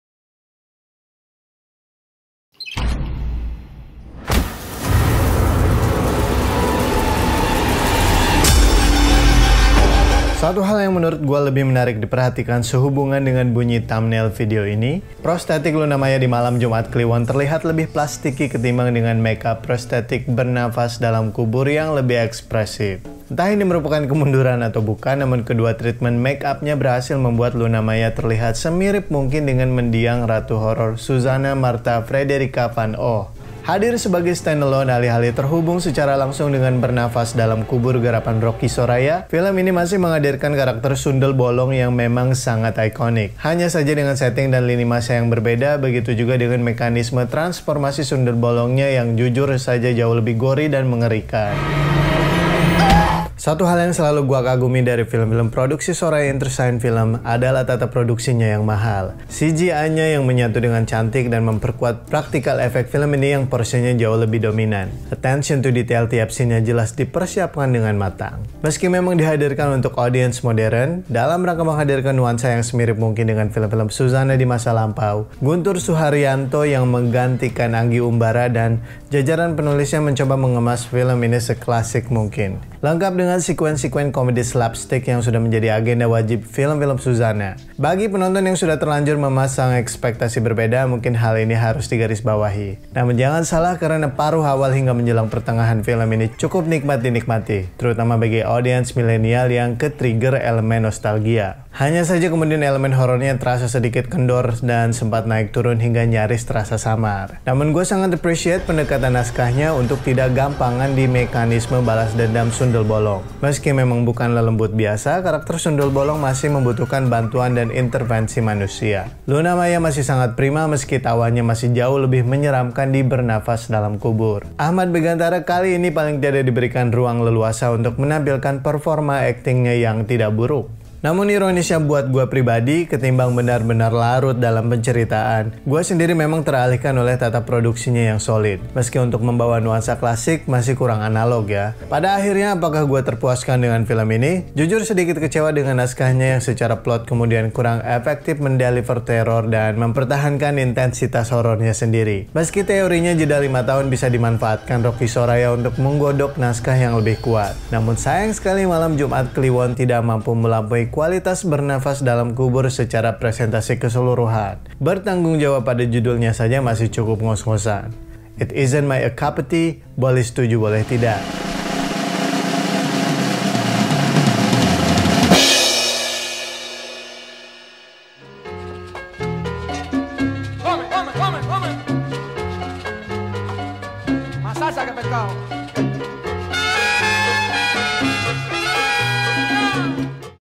Review - Suzzanna Malam Jumat Kliwon (2023)
Free spoiler review SUZZANNA MALAM JUMAT KLIWON (2023)

Sutradara Guntur Soeharjanto
Penulis Tumpal Tampubolon, Ferry Lesmana, Sunil Soraya
Dibintangi Luna Maya, Achmad Megantara, Tio Pakusadewo, Taskya Namya & Sally Marcellina